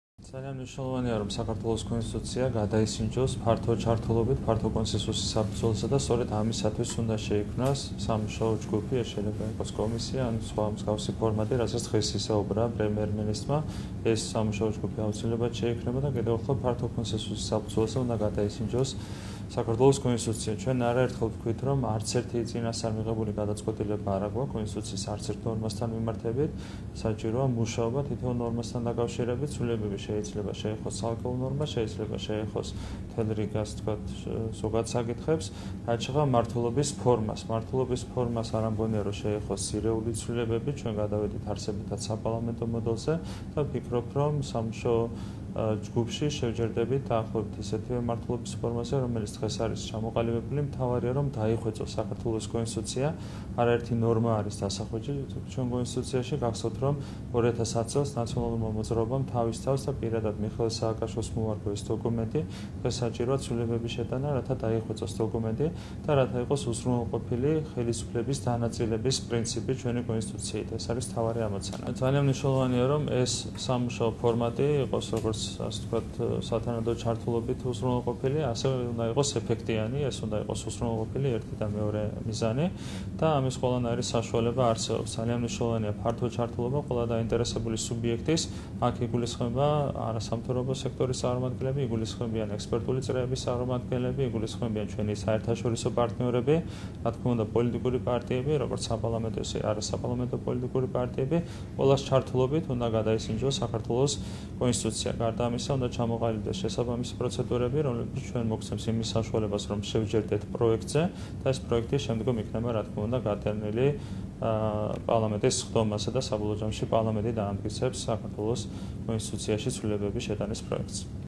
კონსტიტუცია ფართო კონსესუსის საფუძველზე უნდა გადაისინჯოს - ირაკლი კობახიძე
„ძალიან მნიშვნელოვანია, რომ საქართველოს კონსტიტუცია გადაისინჯოს ფართო კონსესუსის საფუძველზე და სწორედ ამისათვის უნდა შეიქმნას სამუშაო ჯგუფი“ - განაცხადა „ქართული ოცნების“ აღმასრულებელმა მდივნმა ირაკლი კობახიძემ. მისივე თქმით, „ქართულ ოცნებას“ კონსტიტუციის ნორმებთან მიმართებით გადაწყვეტილებები წინასწარ არ აქვს მიღებული.